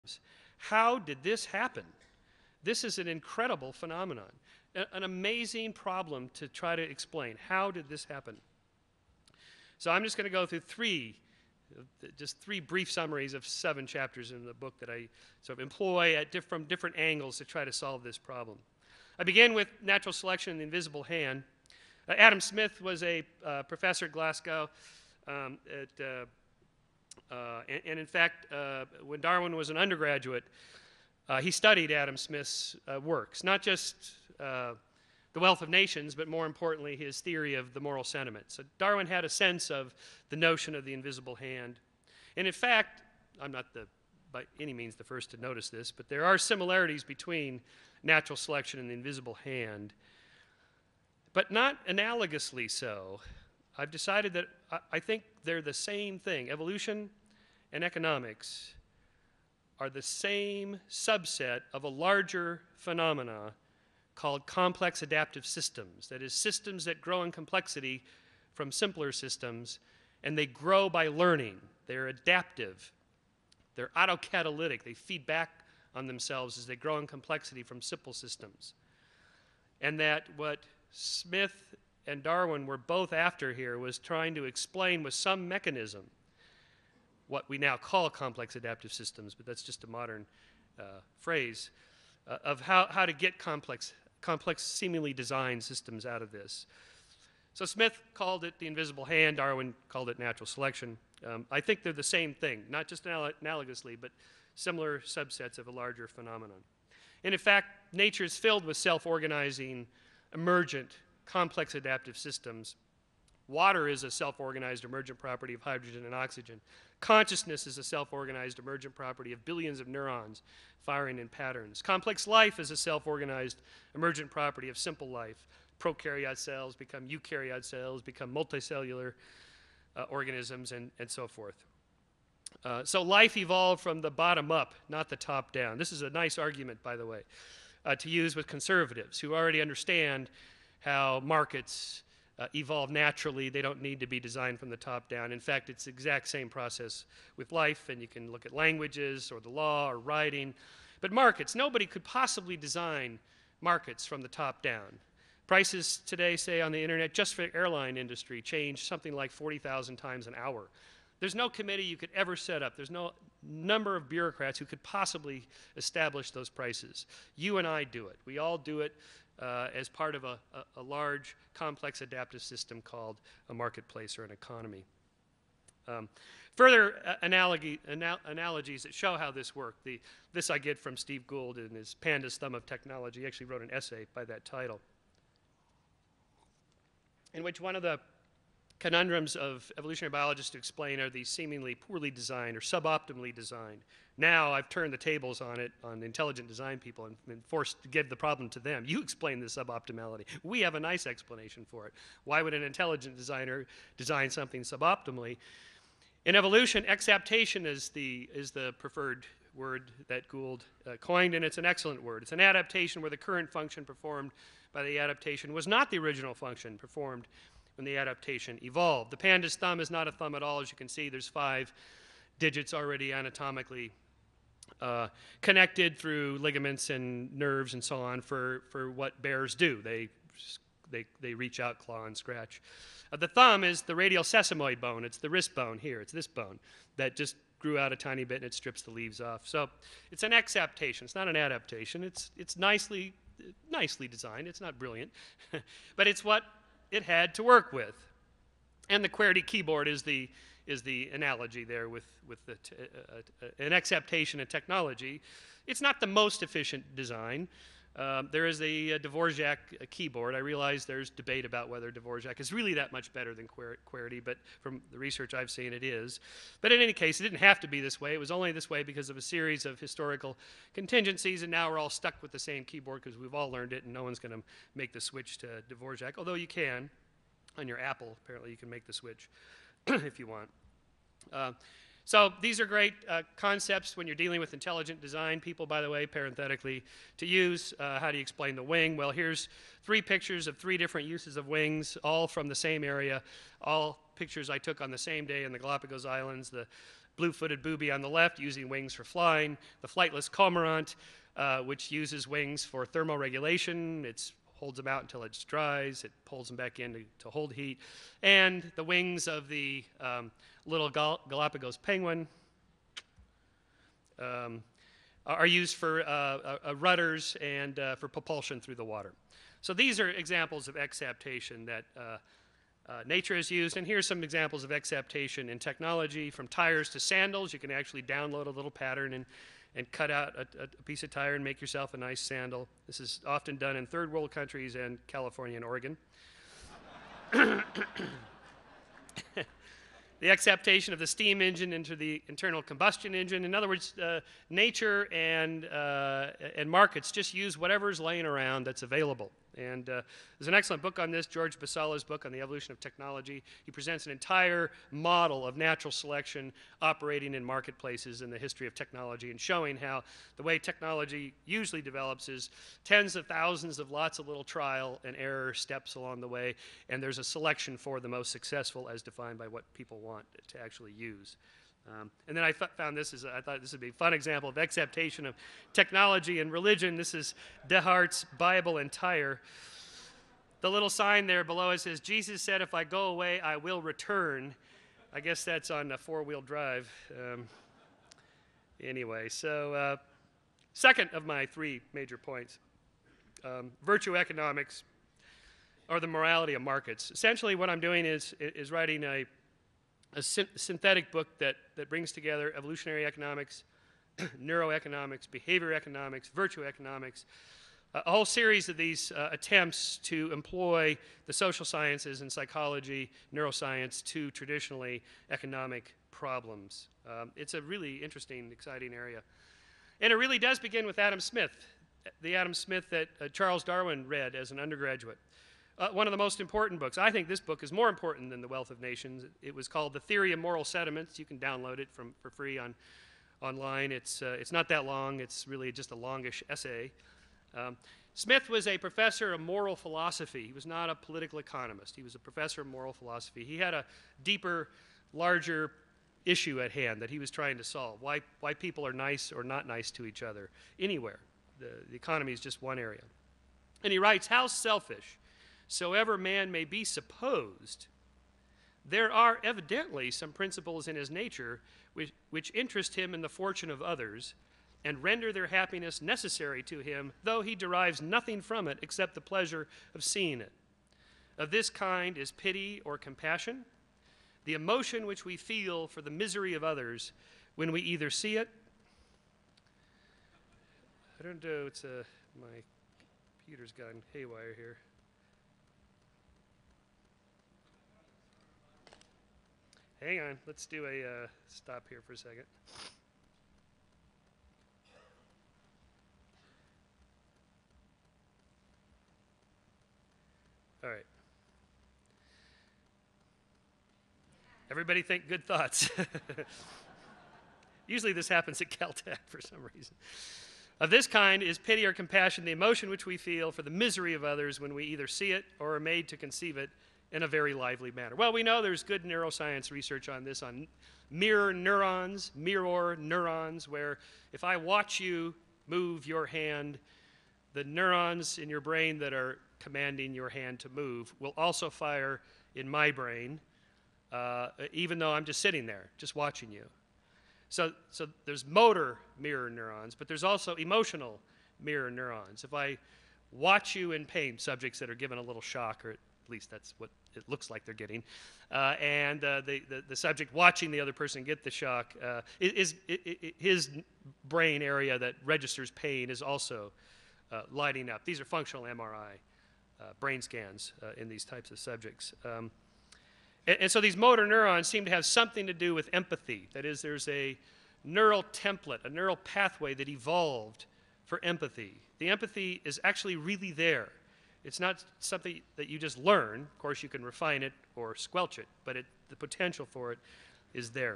Michael Shermer - Evolutionary Economics (part 2)
Lecture by Michael Shermer at "The Amazing Meeting 5" on how the "invisible hand" of the market is to economics as natural selection is to evolution... or rather how they're actually the same (part 2).